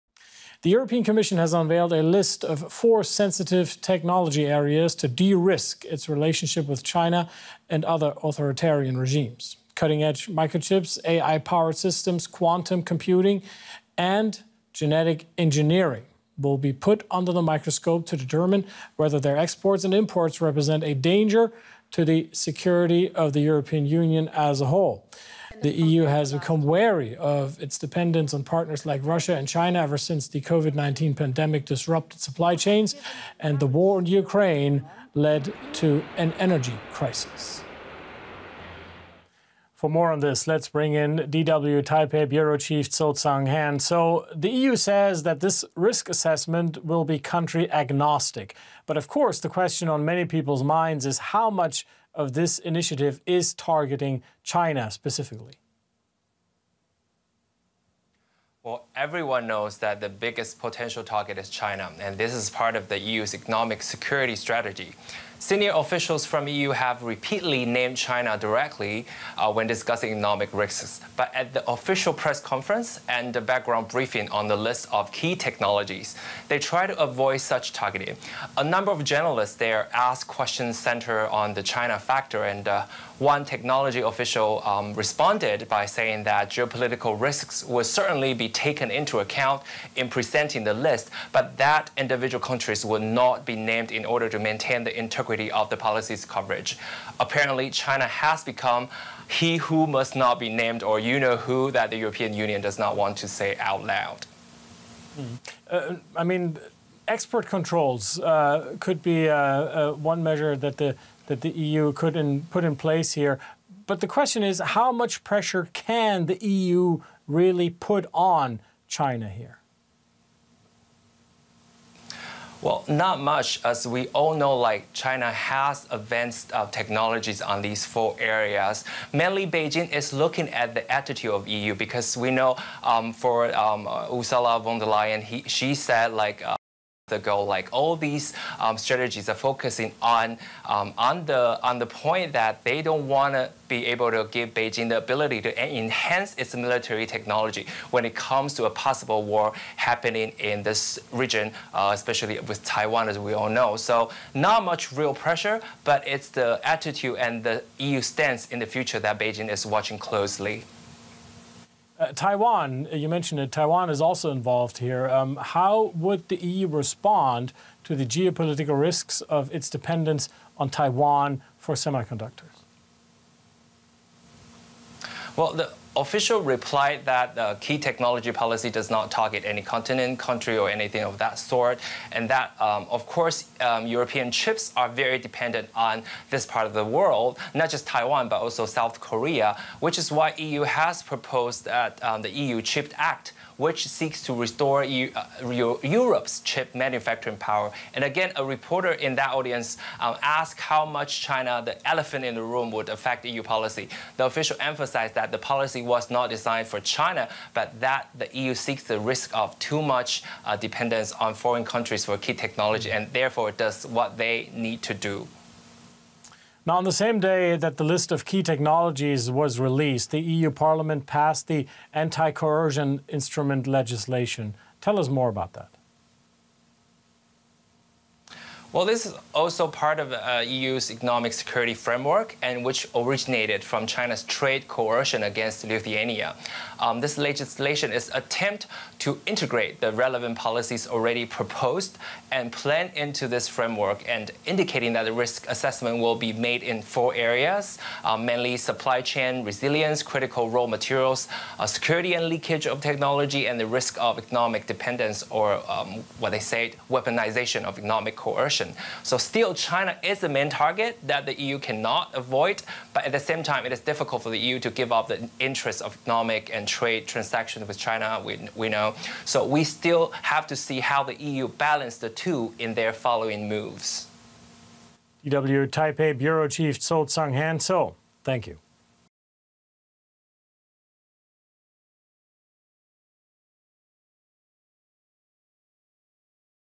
EU unveils sensitive tech areas to de-risk China relationship | DW Business
European Commission unveils a list of four sensitive technology areas to “de-risk” its relationship with China and other authoritarian regimes. Areas include cutting-edge microchips, AI-powered systems, quantum computing and biotech. We speak to tech stocks guru Dan Ives.